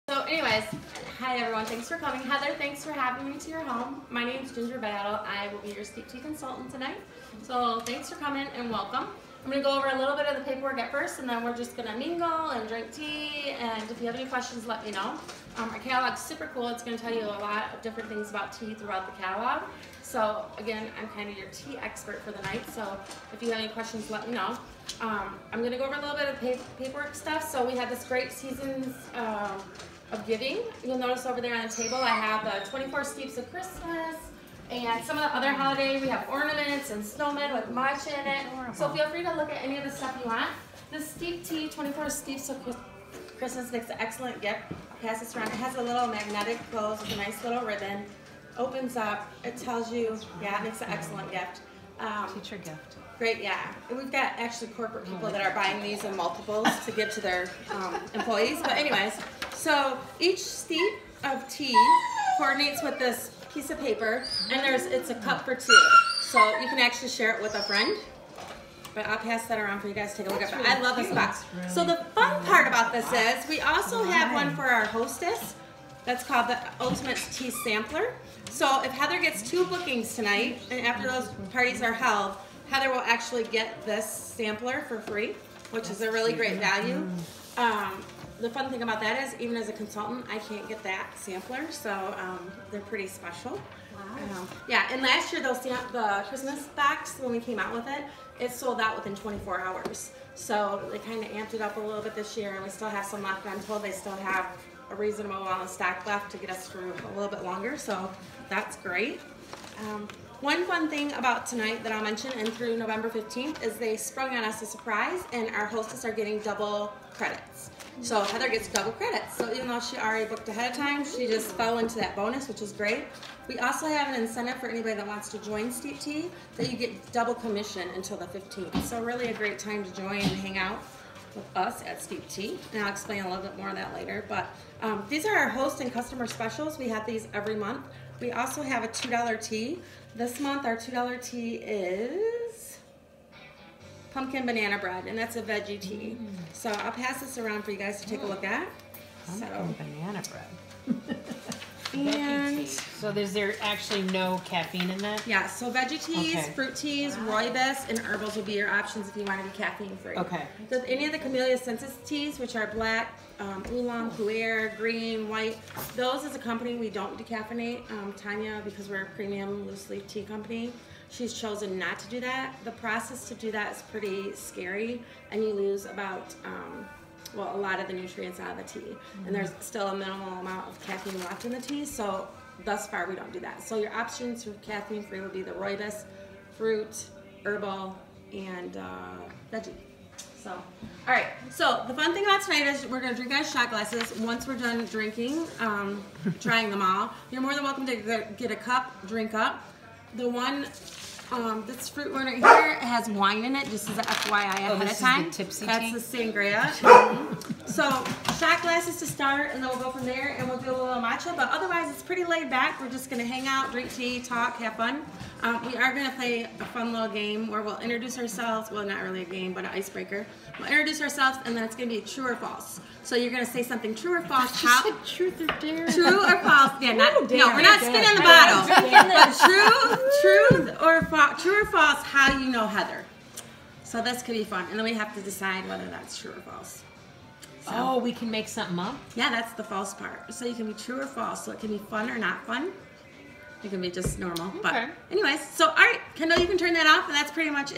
Steeped Tea Pop Up Tea Shop with Ginger Battle
Quick and Easy Steeped Tea Pop Up Tea Shop. 45 min tasting.  With no cheesy games and you don’t have to say your name.  Come late, leave early.  I keep nobody hostage.